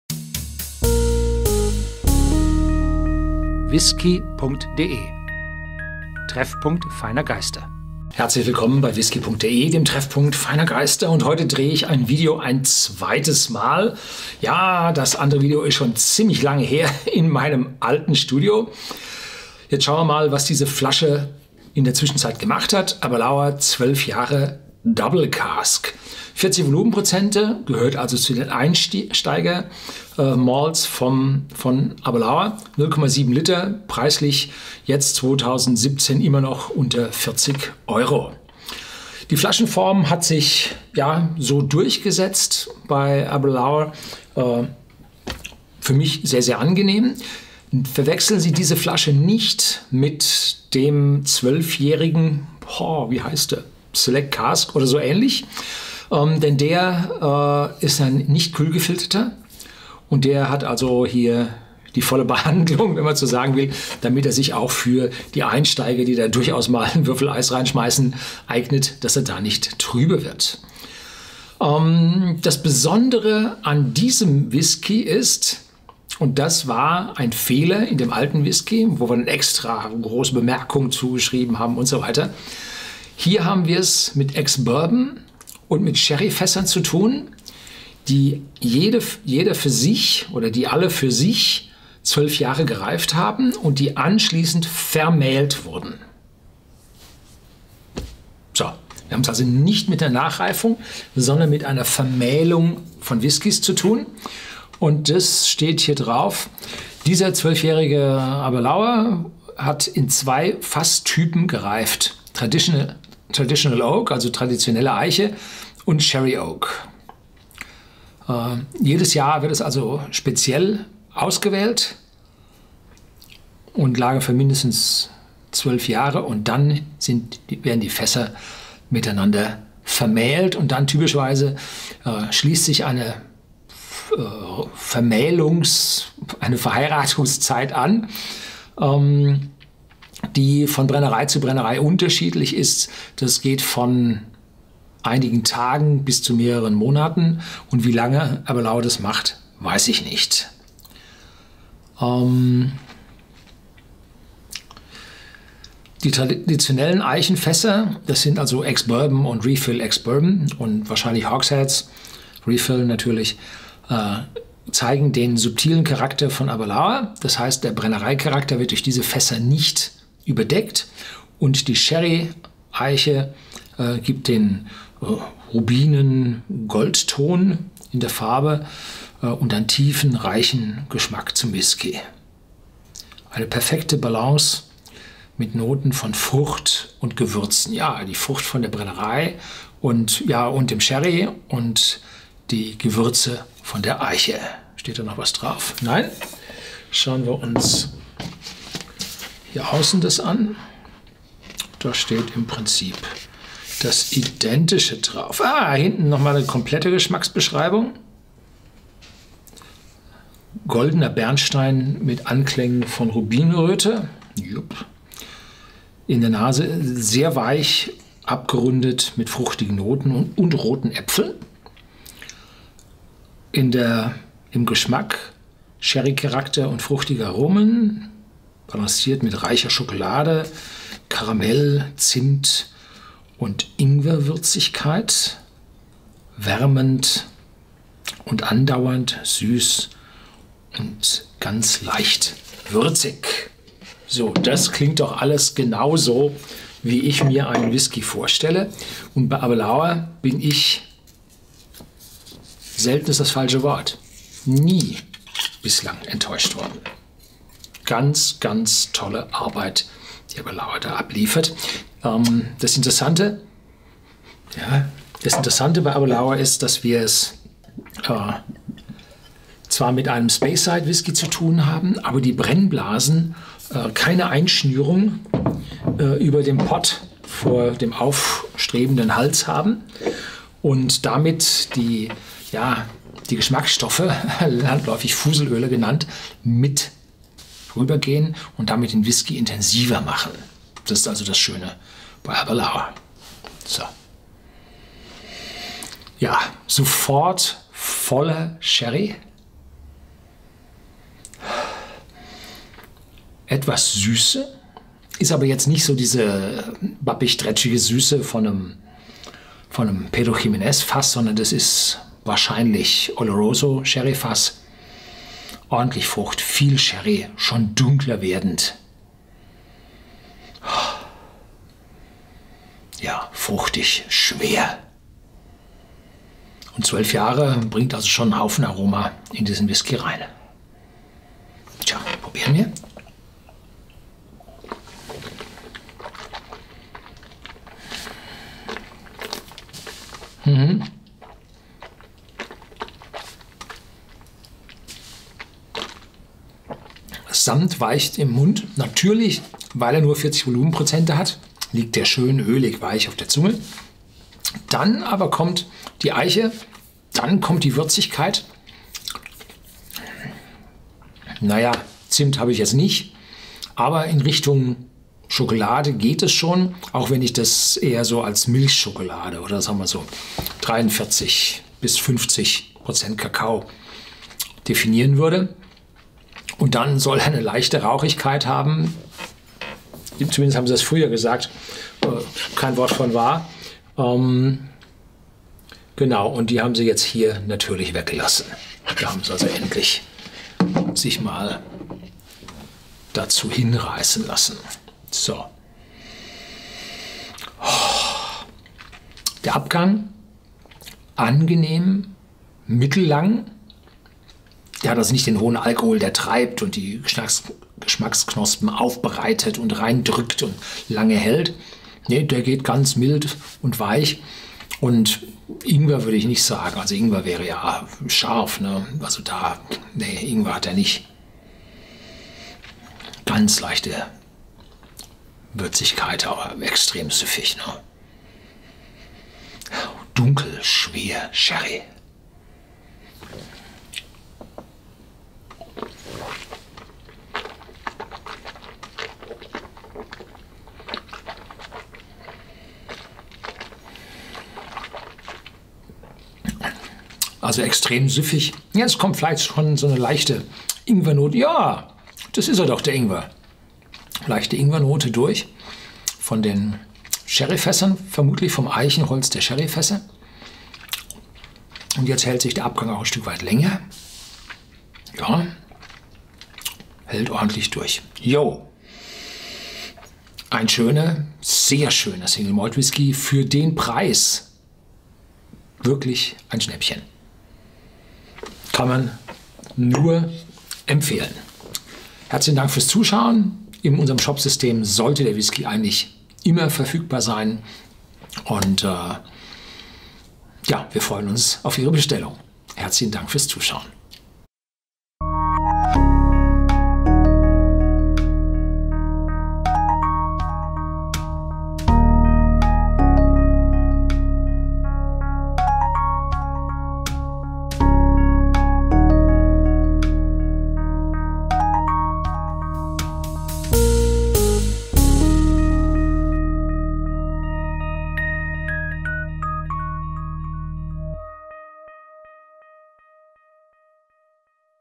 Whisky Verkostung: Aberlour Double Cask 12 Jahre
Nosing 4:54
Whisky.de verkostet den Single Malt Aberlour Double Cask 12 Jahre.